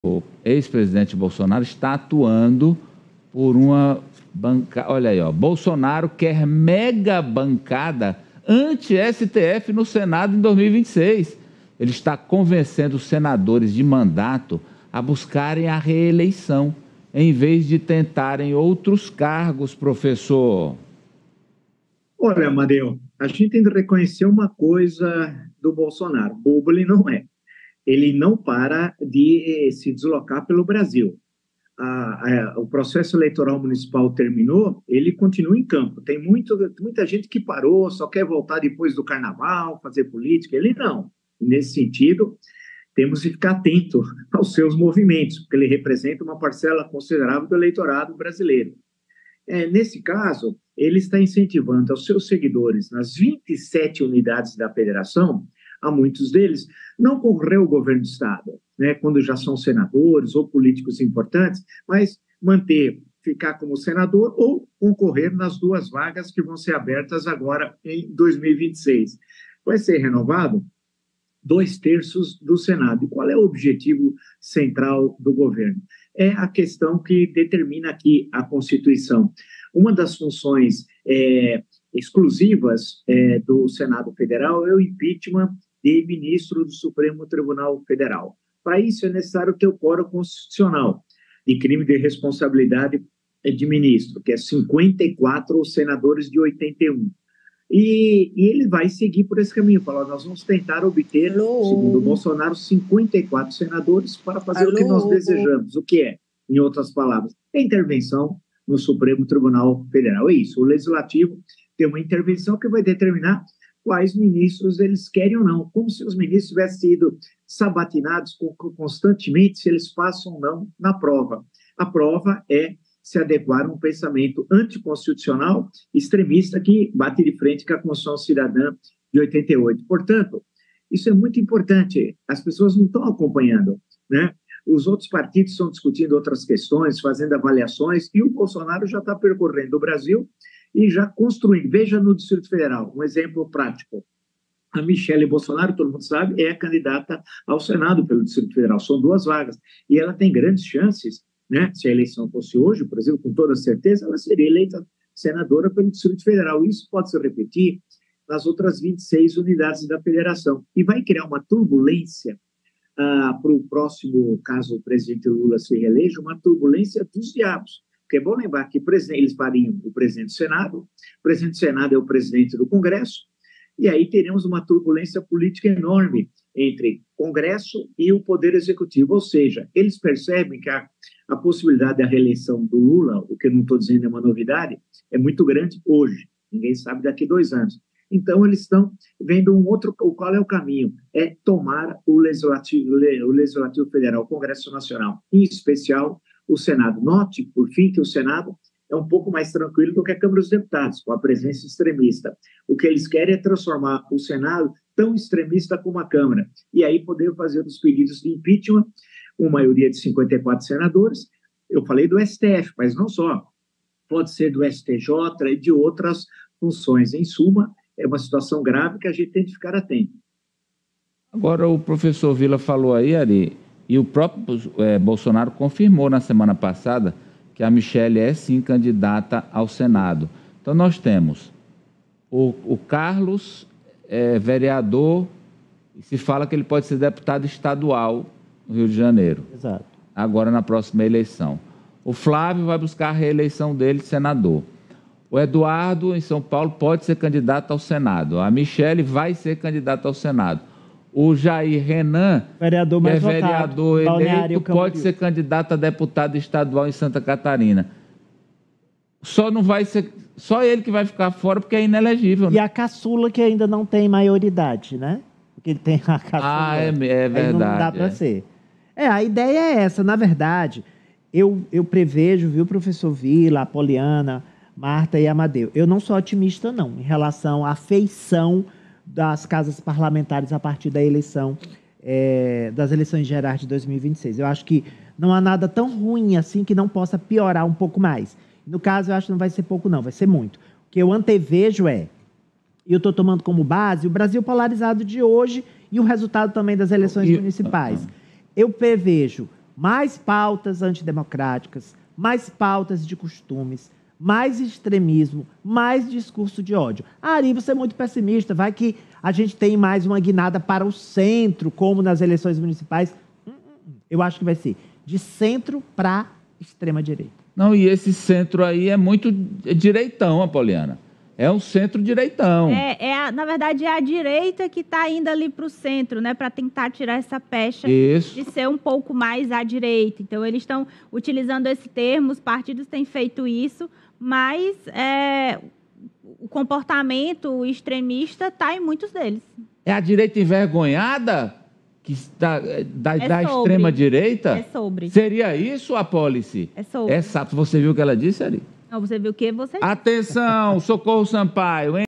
Bolsonaro quer mega bancada anti-STF no Senado em 2026
Bolsonaro tem se articulado para garantir uma bancada expressiva de senadores contrários ao Supremo Tribunal Federal (STF) nas eleições de 2026. O ex-presidente busca formar uma "mega bancada" com parlamentares que compartilhem de suas críticas à atuação da Suprema Corte, visando influenciar futuras decisões e reformas judiciais. Essa estratégia está sendo vista como parte de um esforço maior de Bolsonaro para manter influência na política nacional e moldar o cenário jurídico no país.

Foto: Alan Santos/PR